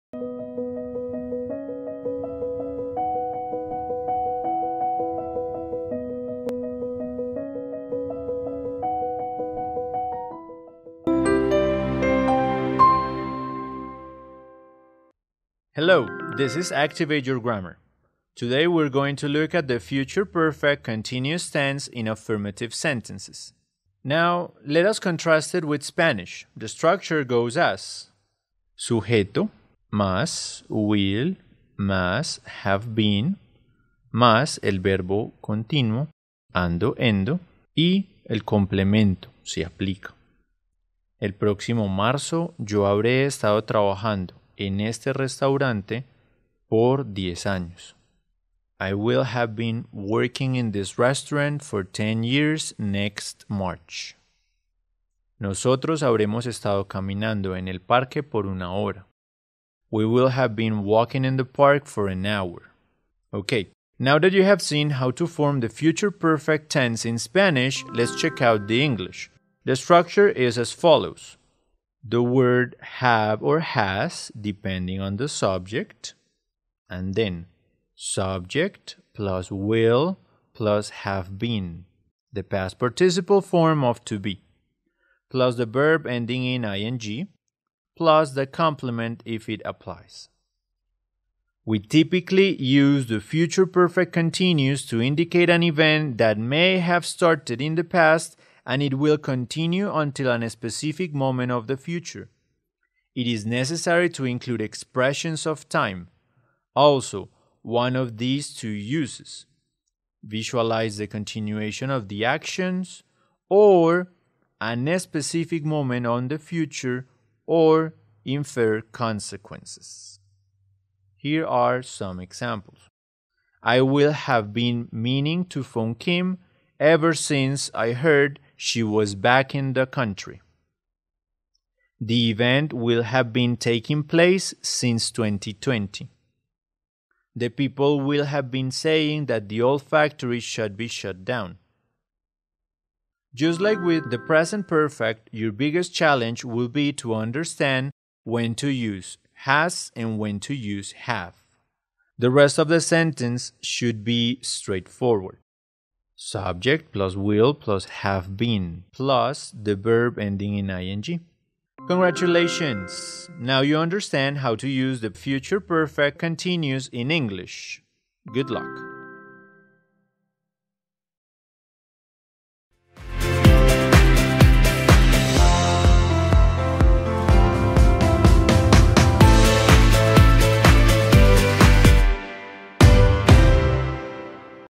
Activate Your Grammar: Future Perfect Continuous Affirmative
Today, we are going to look at the Future Perfect Continuous Tense in affirmative sentences